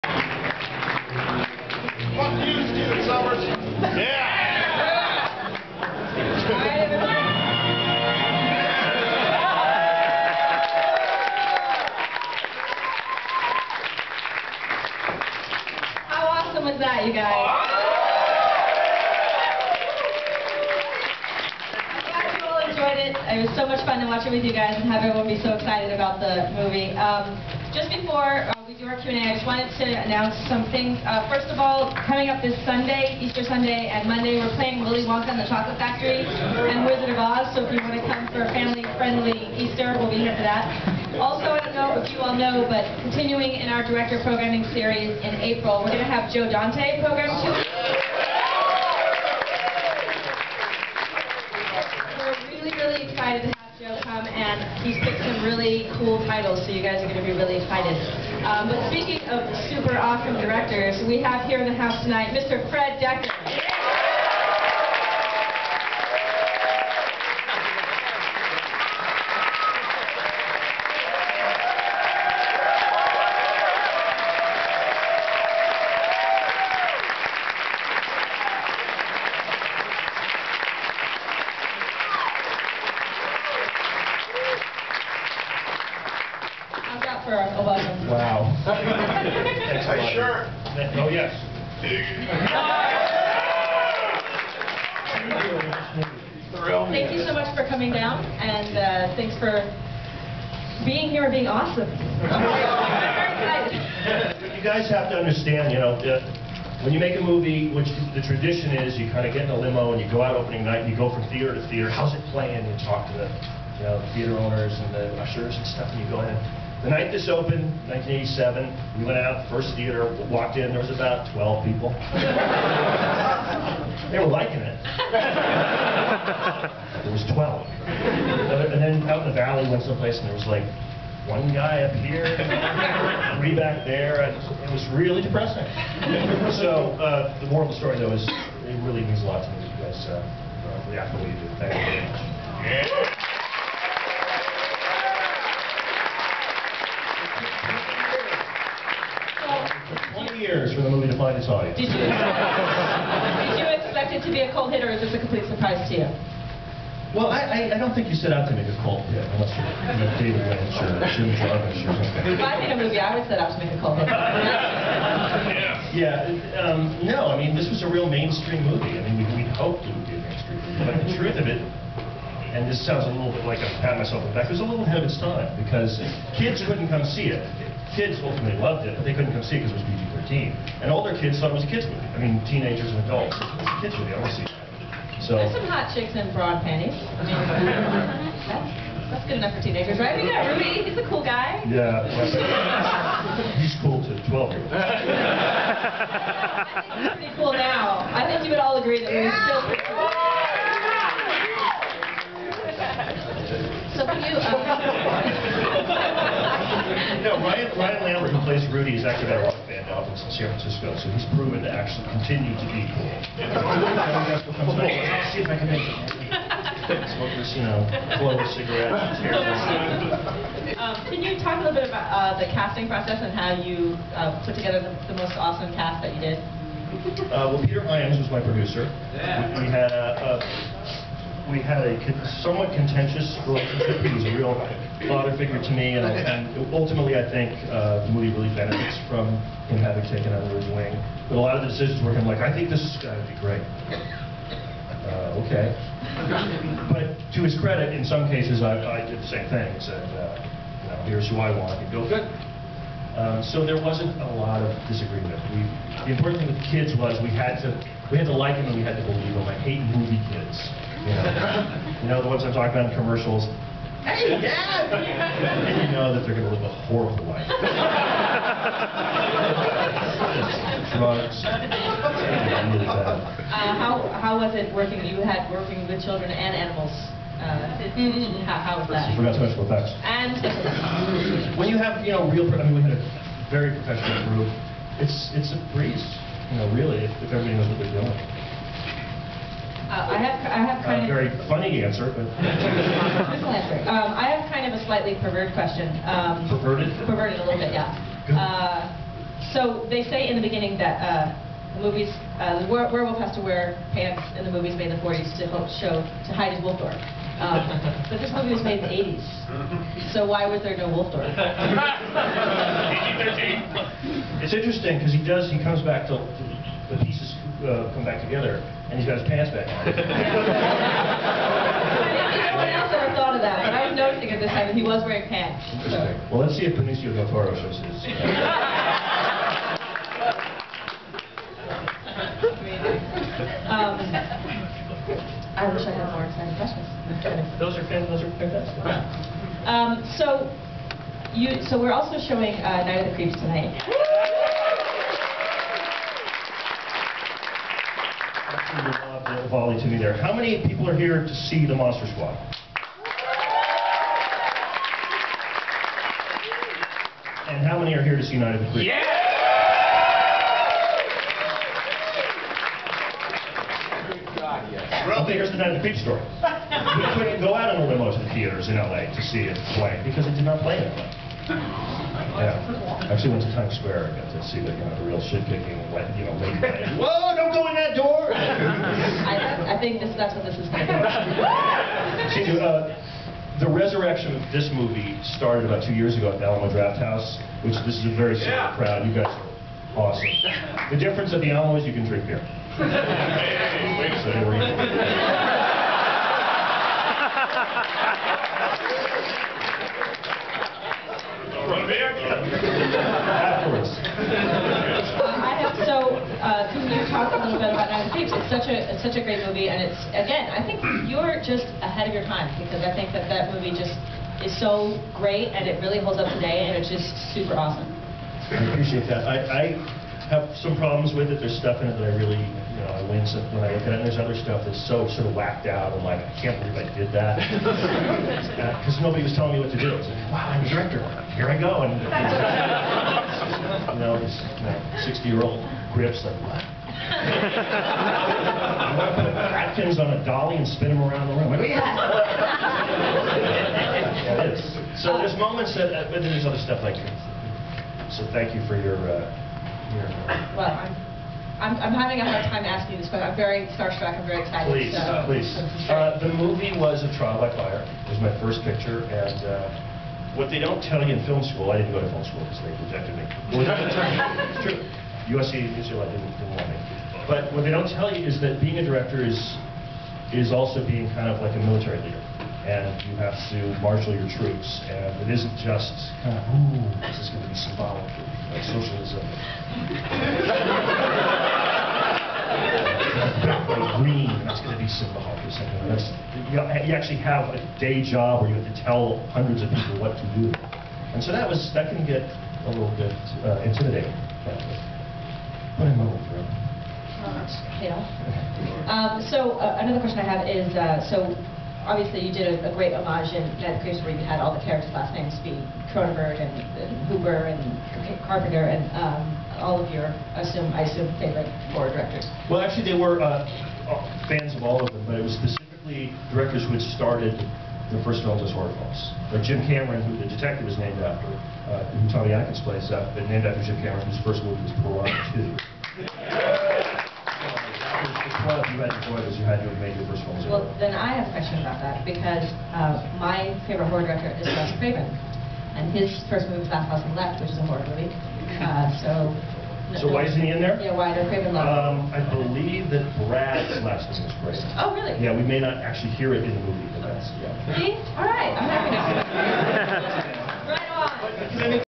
Fred Dekker - Monster Squad/Night of the Creeps Q&A - Pt.1
March 21, 2008 - Fred Dekker speaks during a sold out double feature of Monster Squad (1987)  and Night of the Creeps (1986) at the New Beverly Theater.

To request a DVD release of Night of the Creeps, send mail to:

Sony Pictures Home Entertainment
10202 W. Washington Blvd.